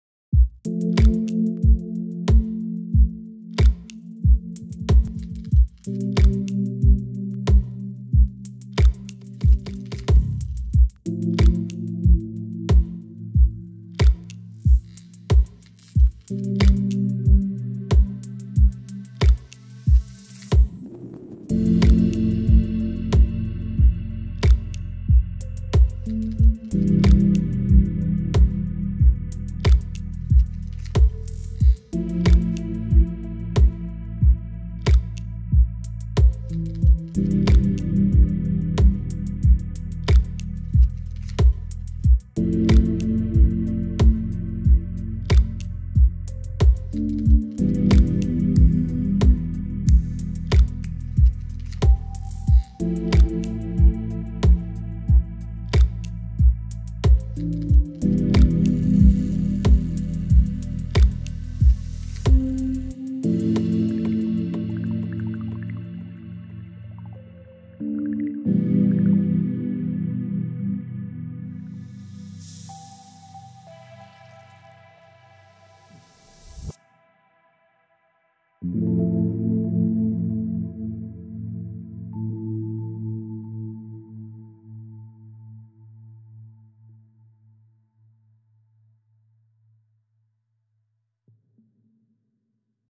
"Afternoon Italian Delight"
A little video I made just to scratch my editing-itch. I was about to make pizza like I usually do, but suddenly got the idea to make a video about it. It turned out pretty intimate and... exotic when my friend put together some sweet ambiance to it.